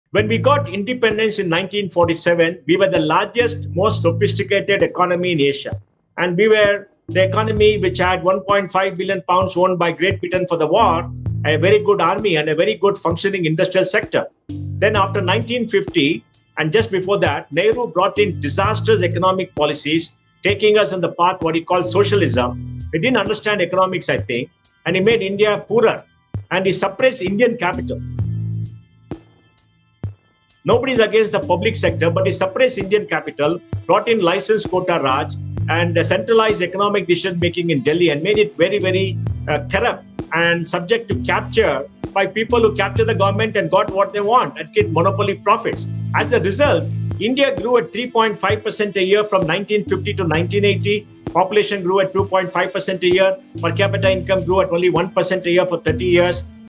Mohandas Pai: "Under Nehru, India grew at only 3.5% a year."
Nehru didn't understand economics. India grew at only 3.5% a year between 1950 & 1980, with per capita income growing at only 1% a year for 30 years: Mohandas Pai